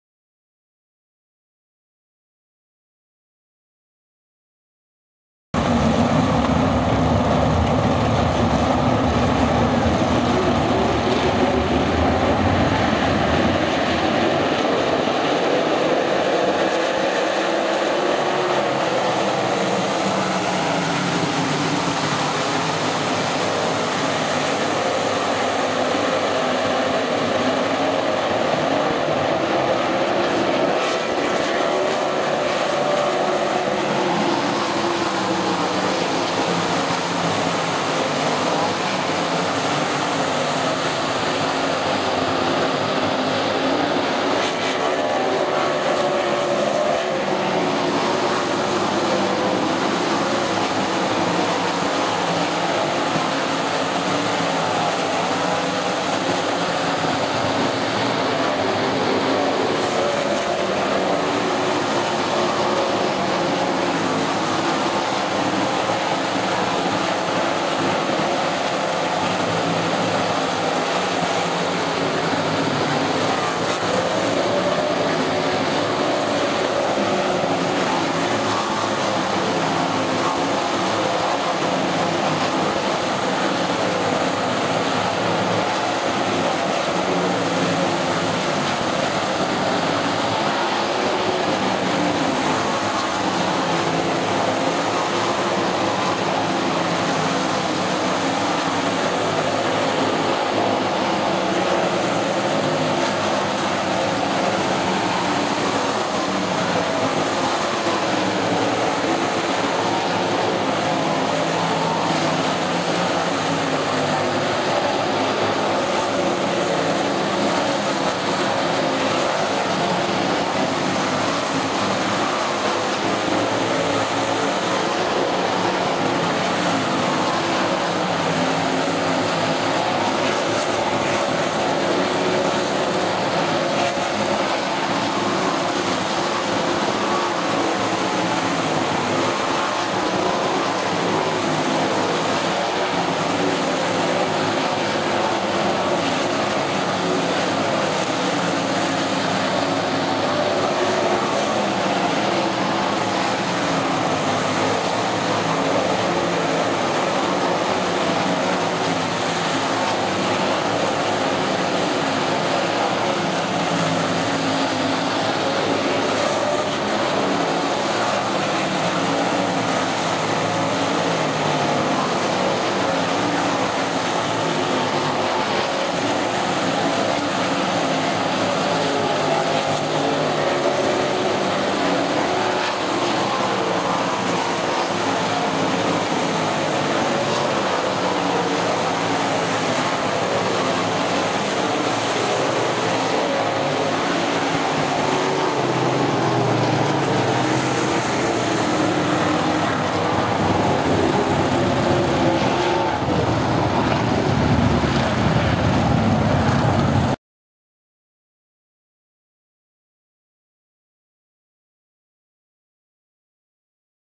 World of Outlaws B-main Eldora
Watch the World of Outlaws at Eldora Speedway 2011.